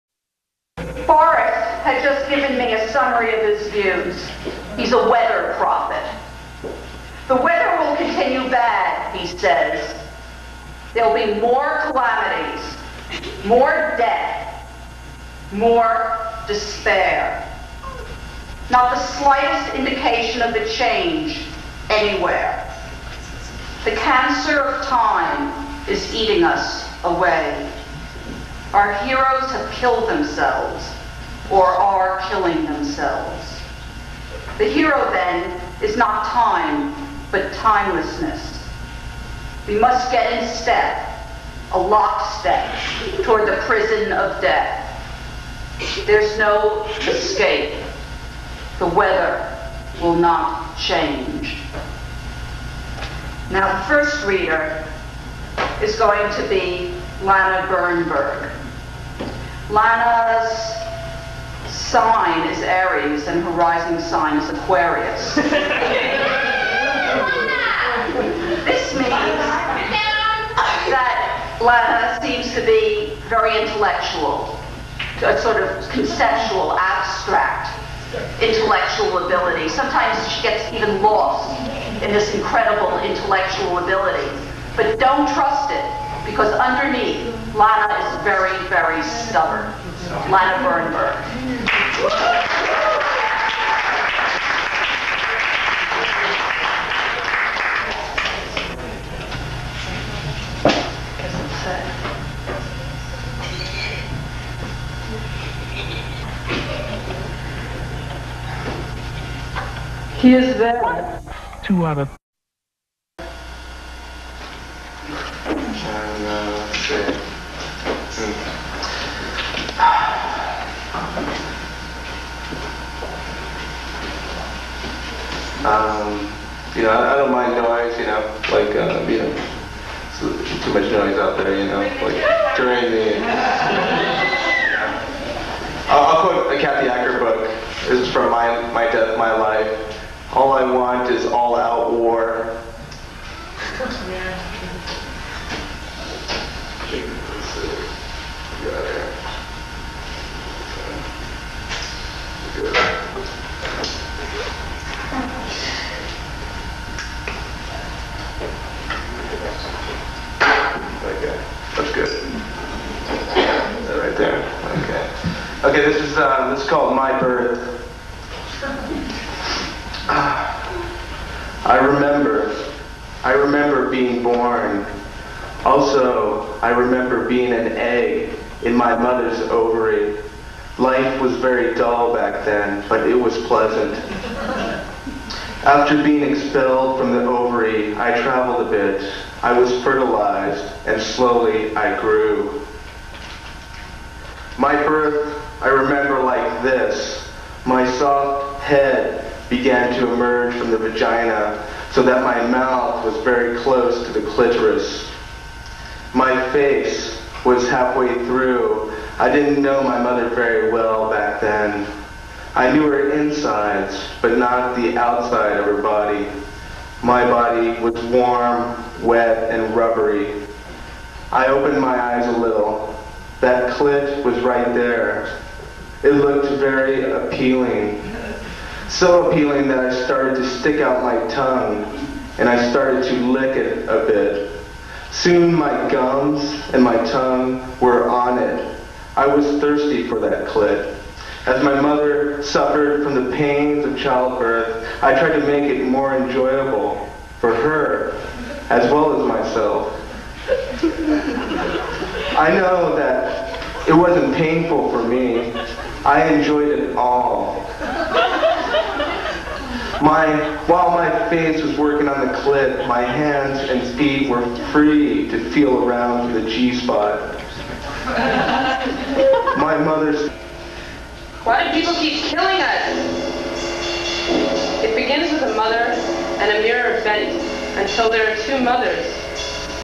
Kathy Acker poetry reading San Francisco 1991
This video is about a Kathy Acker SFAI class poetry reading in San Francisco December 1991. Kathy Acker (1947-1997) w/ Lana Bernberg, Alexander Laurence, and others.

Kathy Acker was a singular figure in late-20th-century Western literature who moved between the avant-garde art and literary scenes of New York, San Francisco, San Diego, Seattle, Paris and London. In her provocative, performative writing, she confronted many issues -- sex, patriarchy, the body, war, colonialism, and much more -- that remain critically relevant to this day.

“Kathy would tell her students at the Art Institute, 'Don’t let anybody tell you how to write …' Kathy apparently didn’t criticize student work, she just gave them permission … The Art Institute was too sterile for her methods, so she held her classes at a bar -- the appropriately gothic Edinburgh Castle. Bob Glück theorizes that students didn’t learn from Kathy, they absorbed her.”